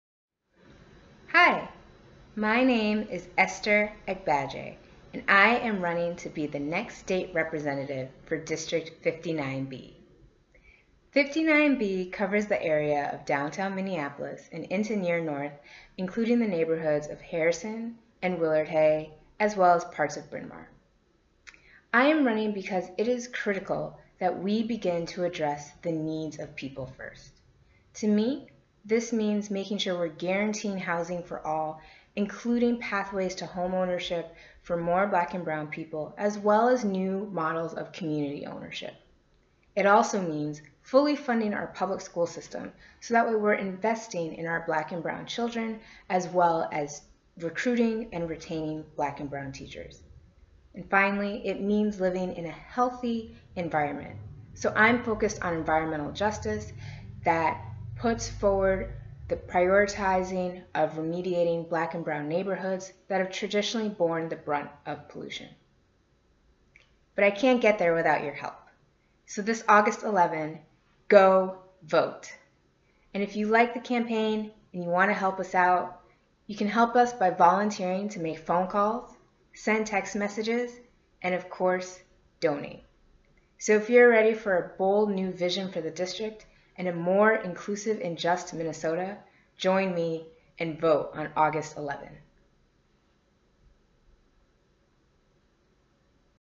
Esther Agbaje House of Representatives 59B
Esther has the values and vision for Minnesota District 59B My name is Esther Agbaje, and I’m running to be the State Representative for District 59B in Minneapolis. The district is the heart of the city, from the vibrant communities of the North Side to the Hennepin County Medical Center and the Mississippi River.